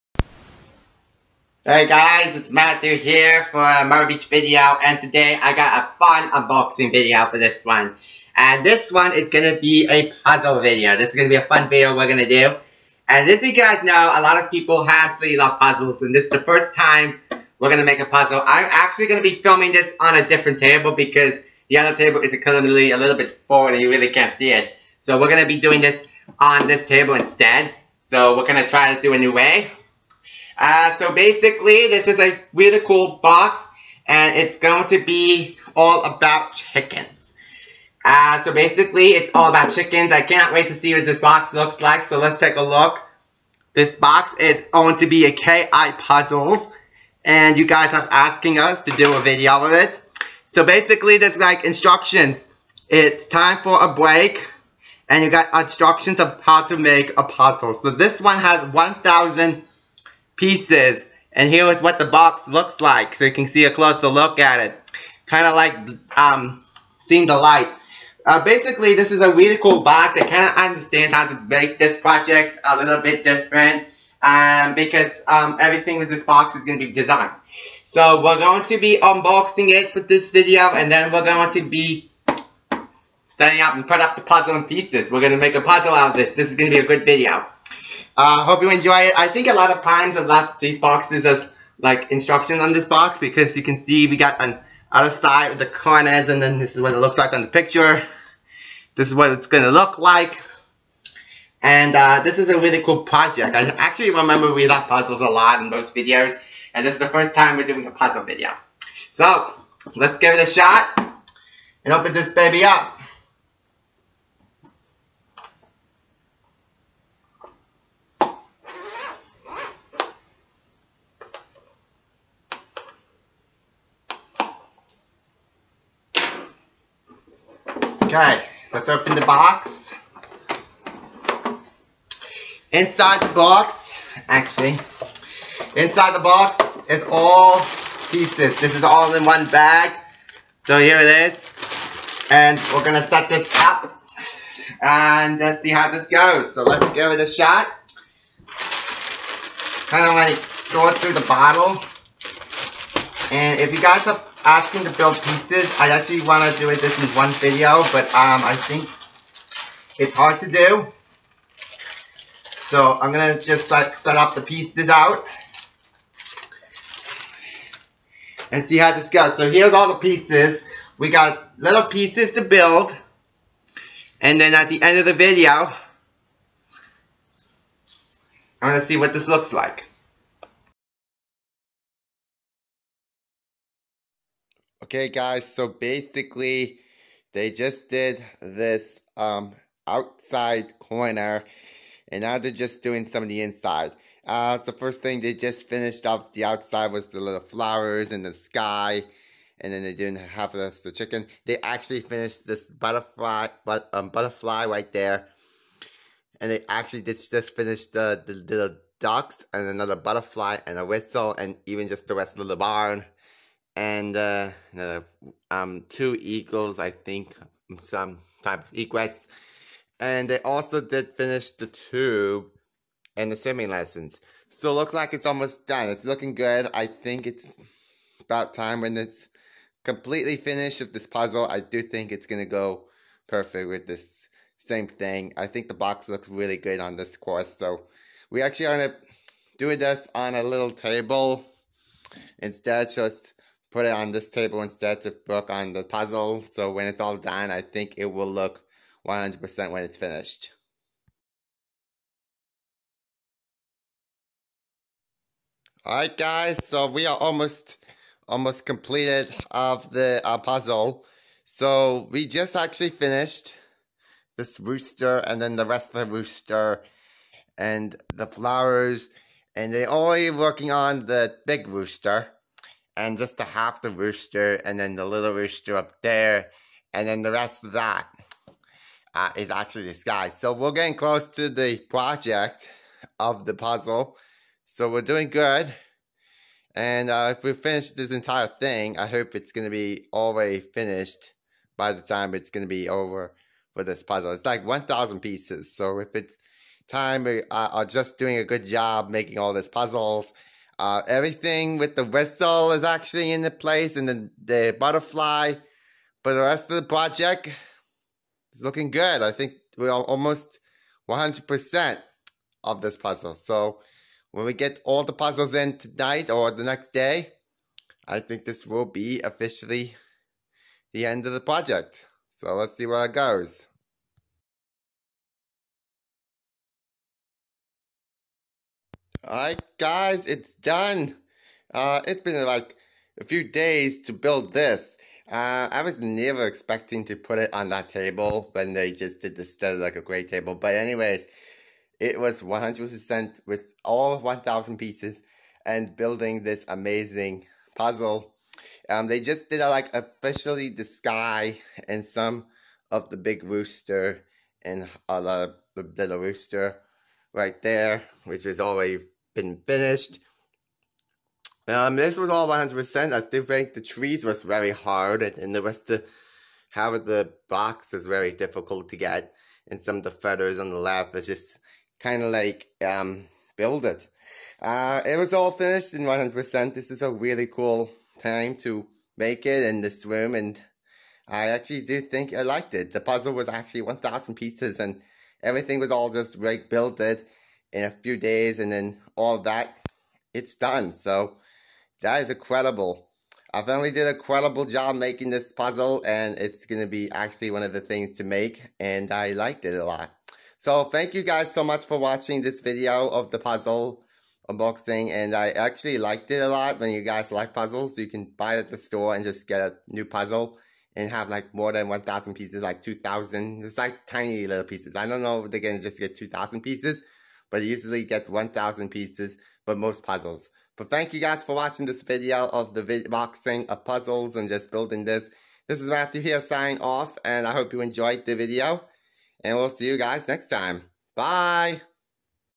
Myrtle Beach - Karen Burke - Swimming Lessons Puzzle Unboxing
I will open up the first puzzle from Karen Burke to check this out. It has 1000 pieces & they will put it all together in days!